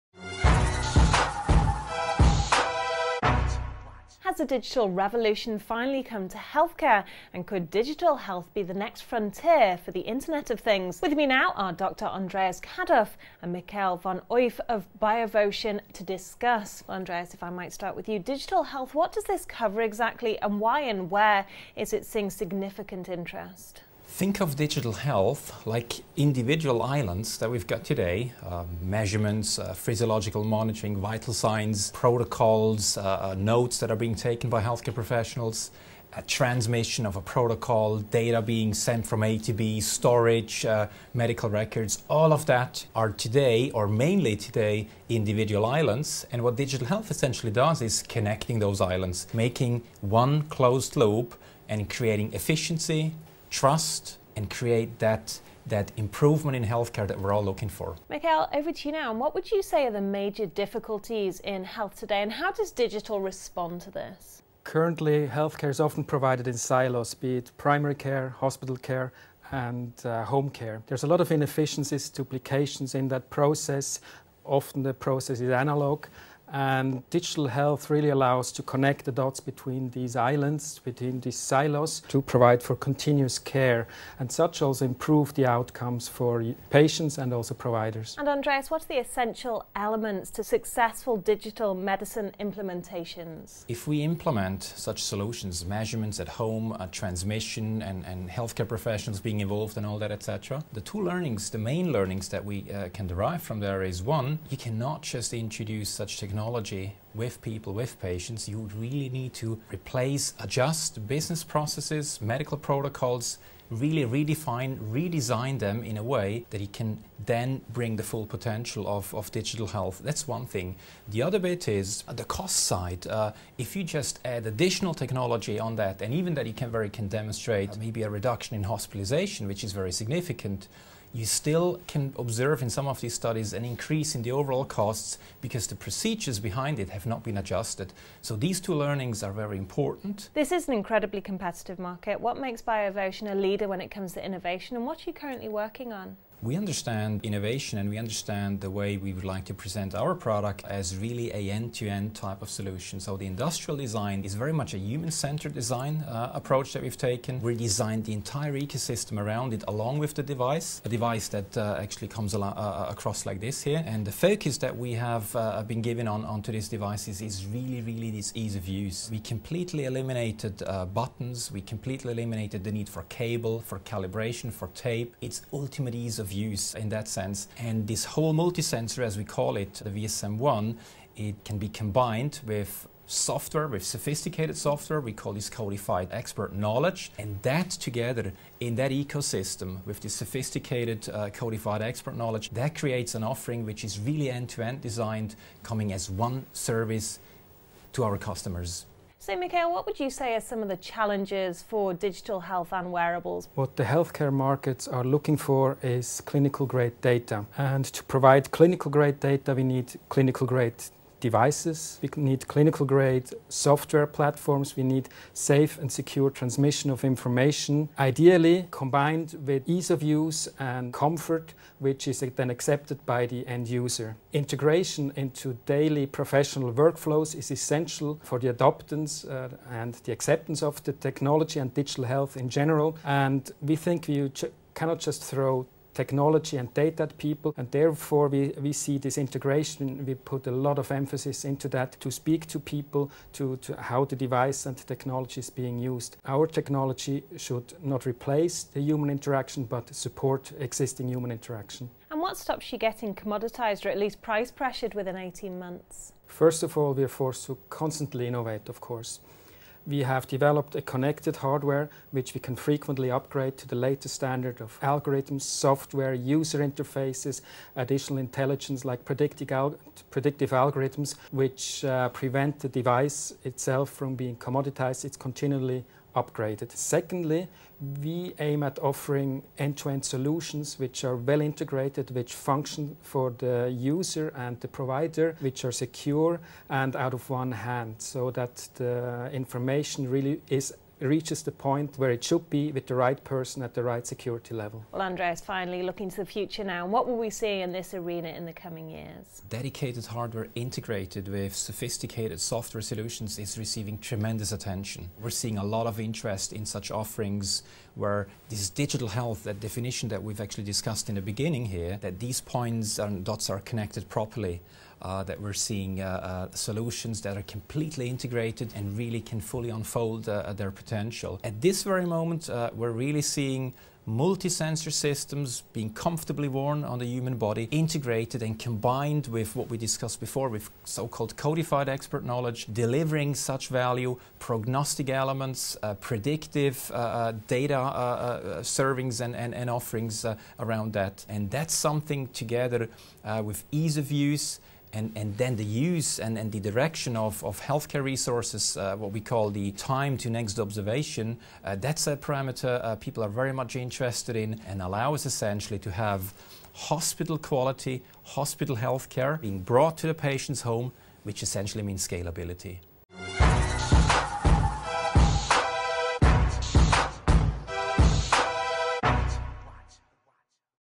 Biovotion connects healthcare silos with VSM1 digital health wearable | European CEO
Has the digital revolution finally come to healthcare? European CEO speaks to Dr Andreas Caduff and Mikael von Euw from Biovotion, who show off Biovotion's VSM1 multi-sensor.